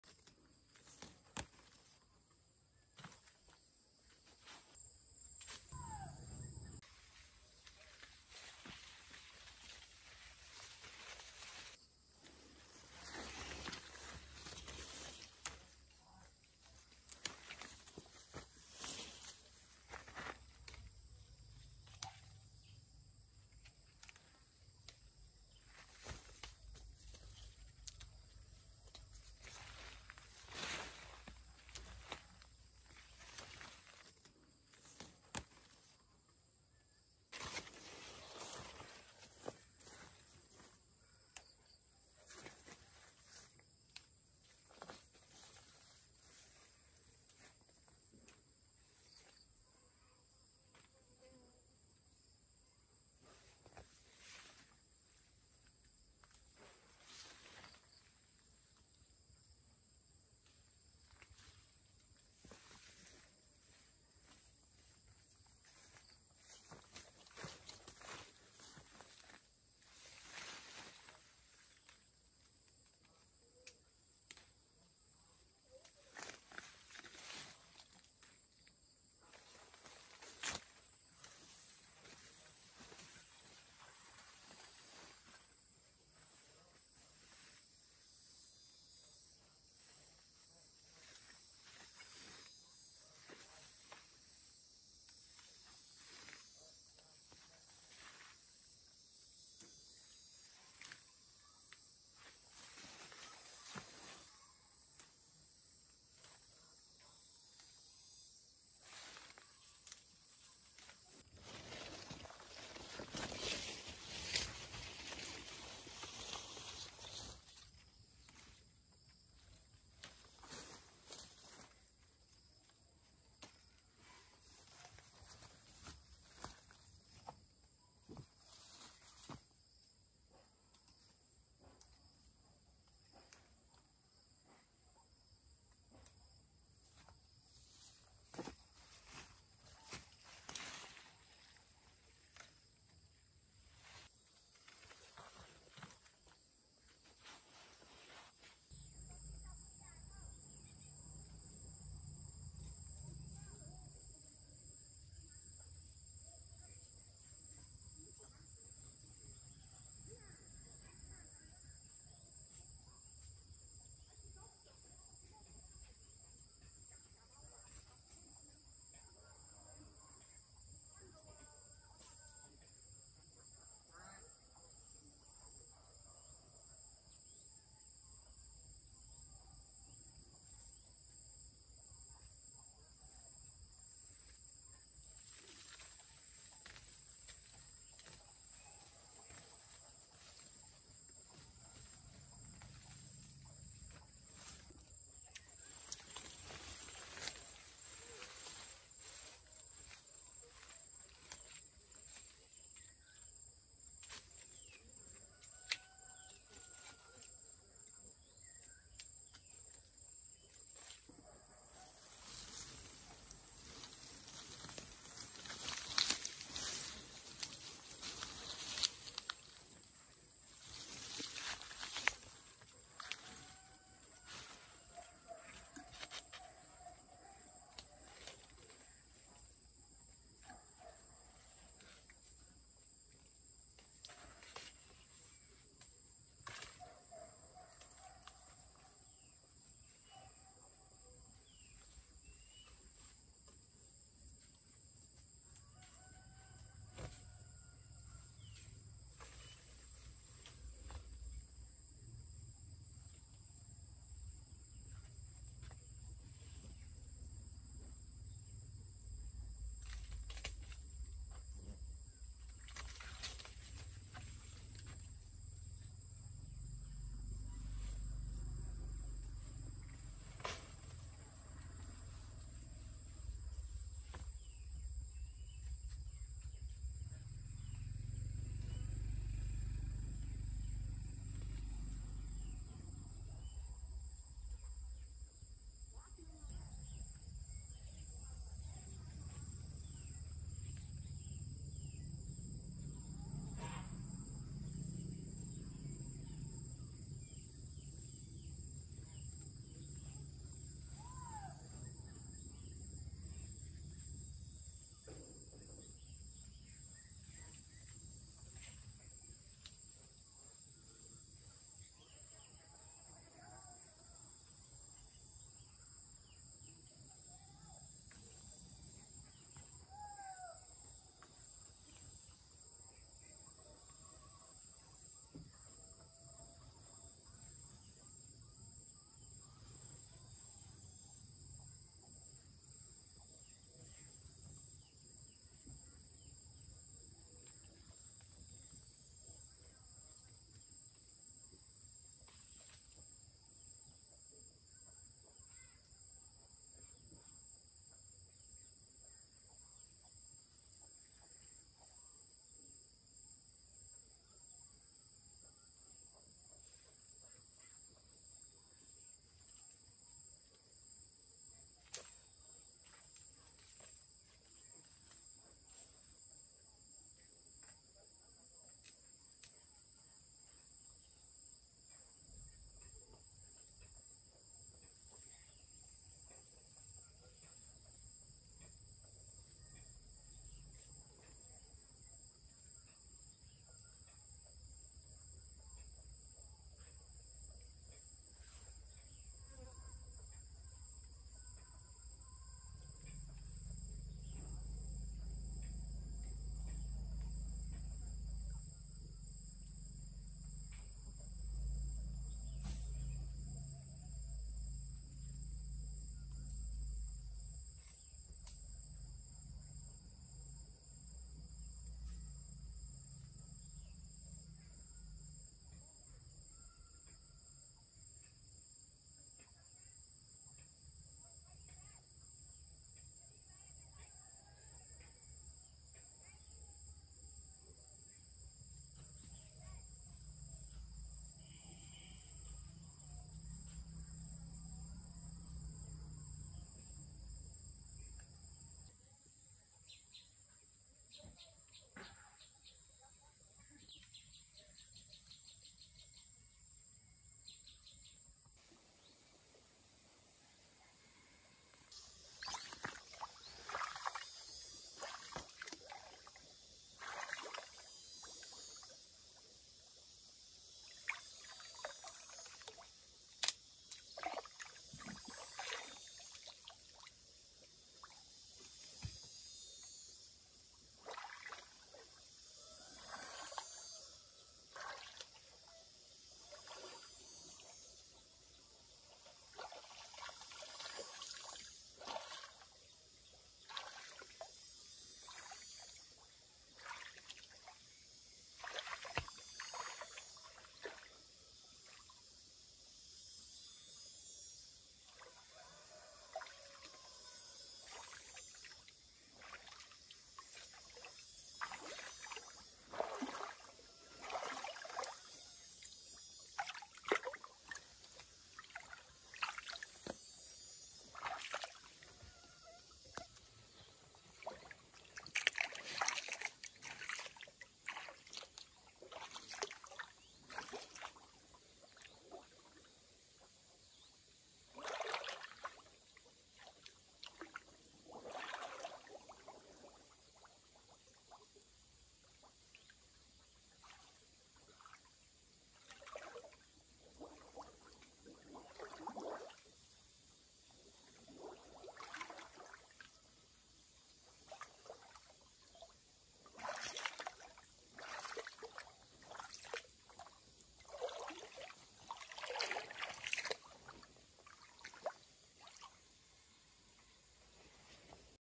Harvesting grapefruit,picking beans and find fish
Welcome to Líp Nhì Tv channel
Harvesting grapefruit,packing beans and find fish